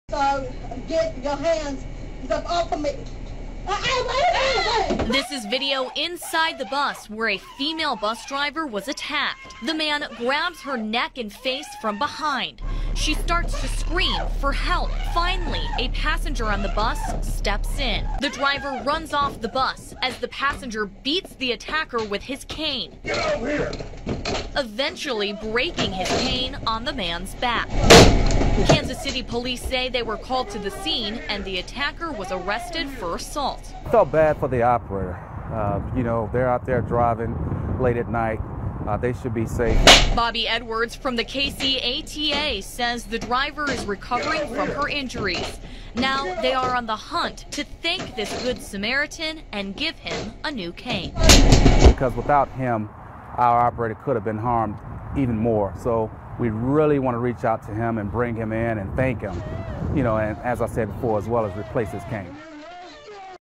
Bus Driver Attack
A bus driver in Kansas City who was attacked was saved by a passenger.